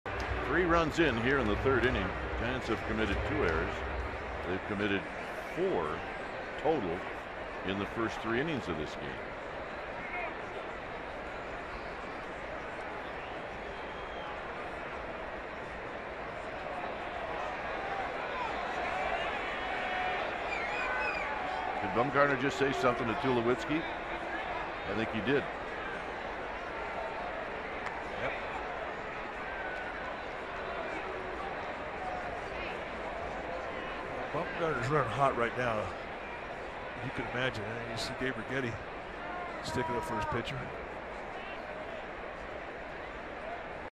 2013/05/18 Tulo, Bumgarner exchange words
SF@COL: Tulo, Bumgarner converse after ump takes ball